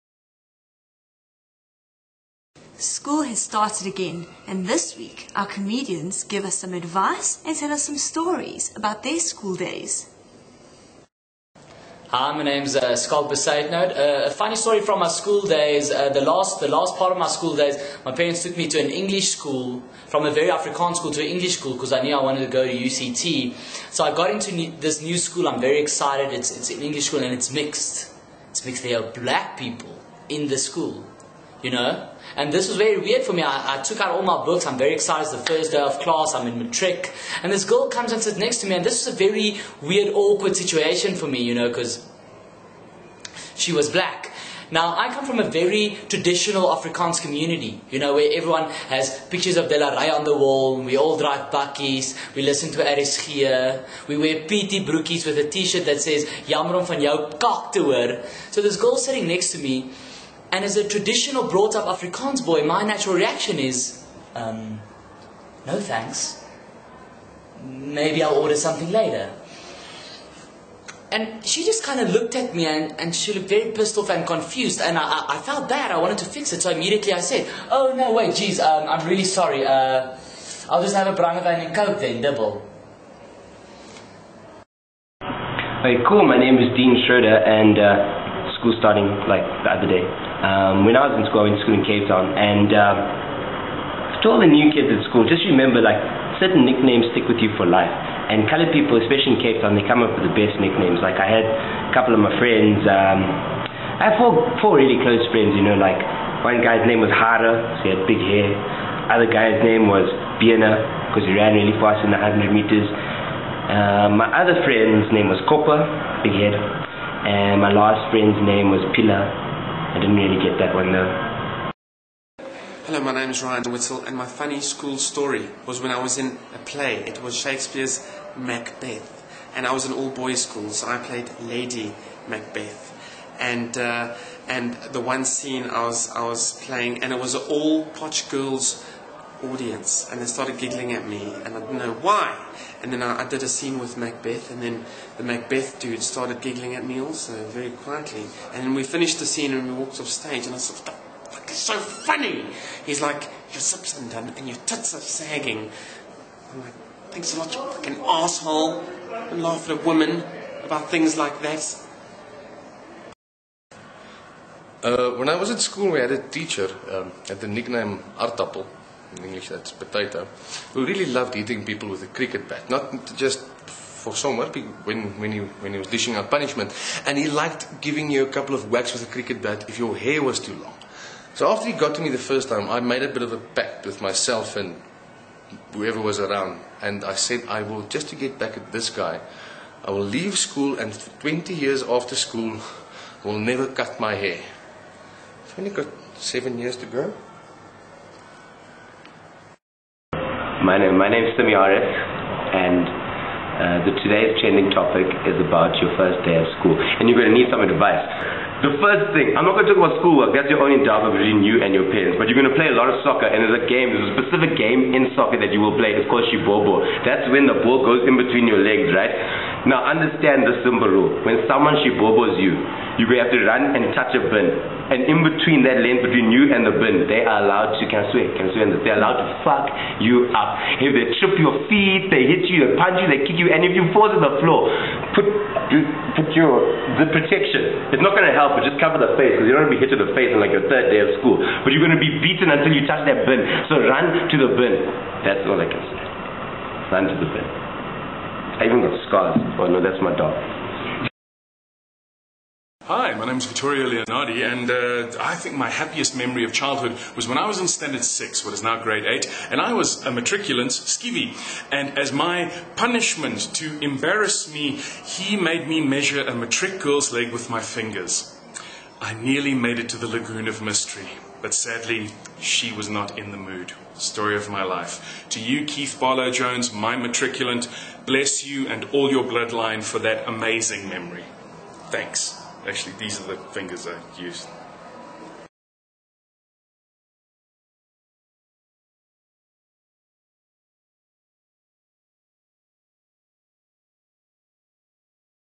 A Story From My School Days
Comedians give us stories and advice from their school days. Featuring Schalk Bezuidenhout, Deen Schroeder, Ryan Whittal, Melt Sieberhagen, Simmi Areff and Vittorio Leonardi. **Apologies for the bad sound on some clips**